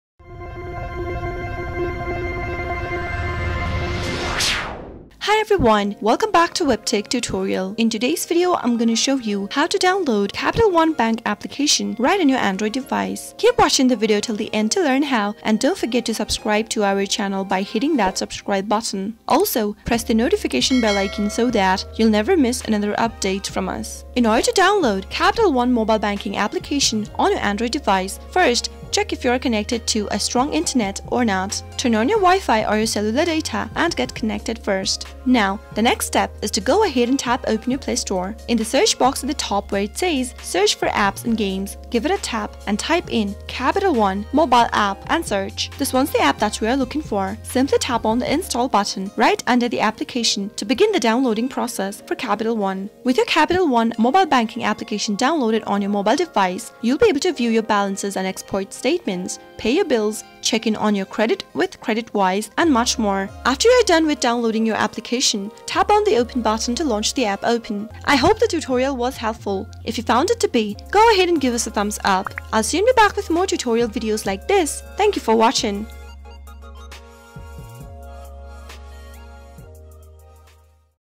How to Download & Install Capital One Bank Mobile Banking App 2022?
In order to download the Capital One mobile banking application on your Android device, first check if you are connected to a strong internet connection or not. After you've finished downloading your app, click the "open" button to begin using it. Turn on your wi-fi or your cellular data and get connected first.

CHAPTERS
0:00 - Intro
0:23 - Download & Install Capital One Bank Mobile Banking App
1:07 - Outro